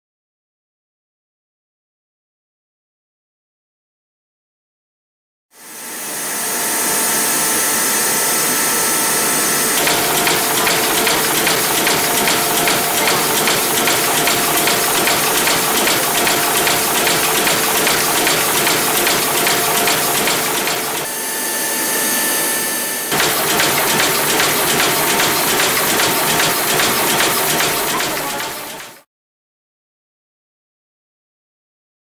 OMBA TA-DE - Wine Capsule Machines Since 1979, Glue metering unit
DOSATORE COLLA
Deposita la colla sulla banda principale. Una siringa pneumatica consente il dosaggio della colla per mezzo di un serbatoio a pressurizzazione regolabile. Il flusso della colla è controllato dal motore di avanzamento nastro, impedendo l’erogazione sotto l’ugello della siringa quando il nastro non scorre.

THE GLUE METERING UNIT
This unit leaves the glue on the main band. A pneumatic syringe allows the glue dosage by means of an adjustable pressurization tank. The glue flow is controlled by the belt feed motor, preventing the glue supply under the syringe nozzle when the belt does not run.